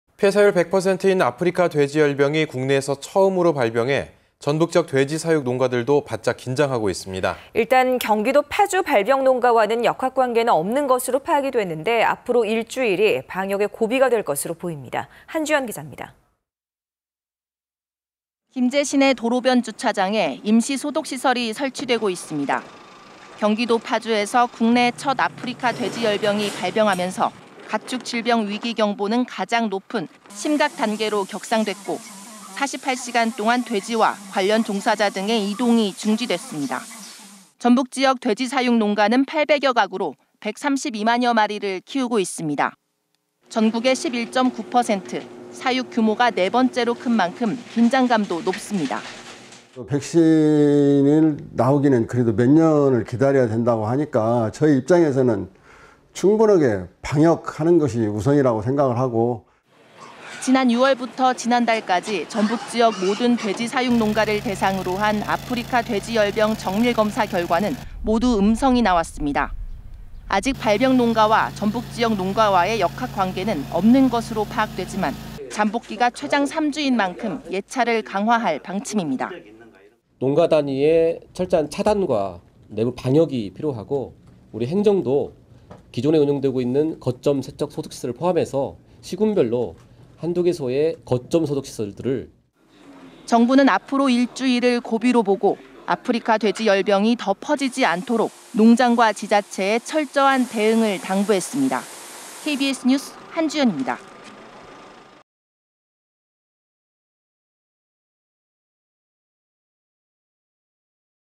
아프리카돼지열병 국내 첫 발병.. 전북도 긴장 2019.09.17(화)
[앵커멘트]

폐사율 백 퍼센트인
아프리카돼지열병이
국내에서 처음으로 발병해
전북지역 돼지 사육 농가들도
바짝 긴장하고 있습니다.
일단 경기도 파주 발병 농가와의
역학 관계는 없는 것으로 파악됐는데,
앞으로 일주일이
방역의 고비가 될 것으로 보입니다.

한주연 기자입니다.

[리포트]

김제 시내 도로변 주차장에
임시 소독 시설이 설치되고 있습니다.

경기도 파주에서
국내 첫 아프리카돼지열병이 발병하면서
가축 질병 위기 경보는
가장 높은 심각 단계로 격상됐고,
48시간 동안 돼지와 관련 종사자 등의
이동이 중지됐습니다.

전북지역
돼지 사육 농가는 8백여 호로,
백32만여 마리를 키우고 있습니다.

전국의 11.9 퍼센트,
사육 규모가 네 번째로 큰 만큼
긴장감도 높습니다.

[인터뷰]
이웅열 / (사)대한한돈협회 전북협의회장
"백신이 나오려면 몇 년을 기다려야 한다고 하니까 저희 입장에서는 충분히 방역하는 게 우선이라고 생각하고..."

지난 6월부터 지난달까지
전북지역 모든 돼지 사육 농가를 대상으로 한
아프리카돼지열병 정밀 검사 결과는
모두 음성이 나왔습니다.

아직 발병 농가와
전북지역 농가와의 역학 관계는
없는 것으로 파악되지만,
잠복기가 최장 3주인 만큼
예찰을 강화할 방침입니다.

[인터뷰]
최재용 / 전라북도 농축수산식품국장
"농가 단위의 철저한 차단과 내부 방역이 필요하고 행정도 기존에 운영하고 있는 거점 세척소독시설을 포함해서 시군별로 한두 개소의 거점 소독시설들을..."

정부는
앞으로 일주일을 고비로 보고,
아프리카돼지열병이 더 퍼지지 않도록
농장과 지자체에
철저한 대응을 당부했습니다.

KBS뉴스, 한주연입니다.